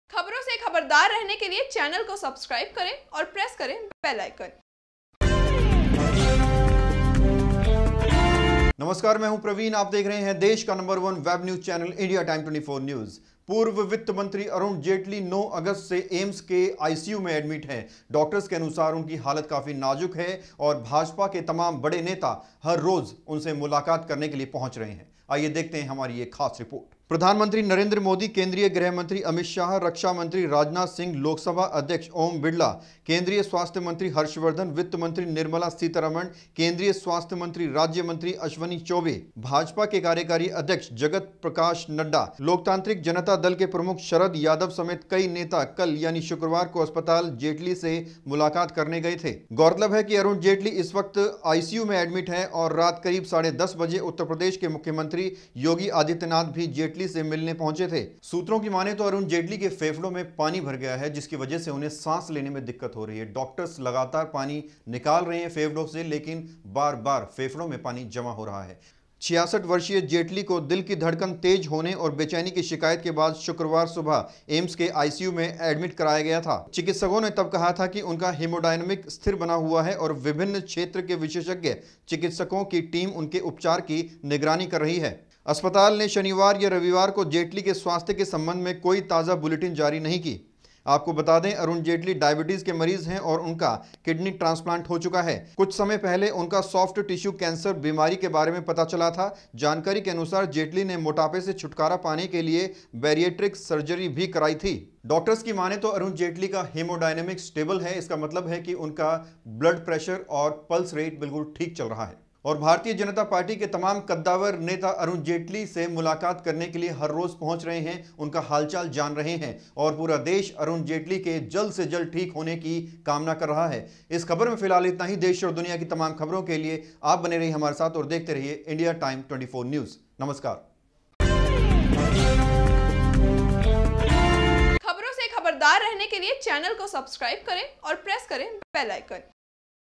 अरुण जेटली की हालत नाजुक, ये सभी नेता मिलने पहुंचे /Indiatime24news
जेटली के फेफड़ों में पानी जमा हो रहा है, जिसकी वजह से उन्हें सांस लेने में परेशानी हो रही है। डॉक्टर्स बार बार उनके फेफड़ों से पानी निकल रहे हैं  लेकिन उनमें फिर से पानी जमा हो रहा है।  डॉक्टर्स की माने तो  अरुण जेटली का हेमडयनमिक स्टेबल है! इसका मतलब है कि उनका ब्लड प्रेशर और पल्स रेट ठीक से काम कर रही है